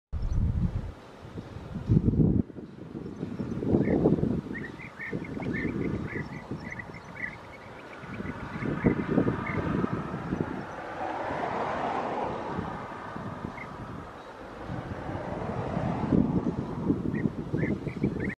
2022 26 06 BEE EATER ДЕТИШКИ ВЫГЛЯДЫВАЮТ ИЗ ОКОШКА ПОСЛЕ КОРМЕЖКИ
Хедера Израиль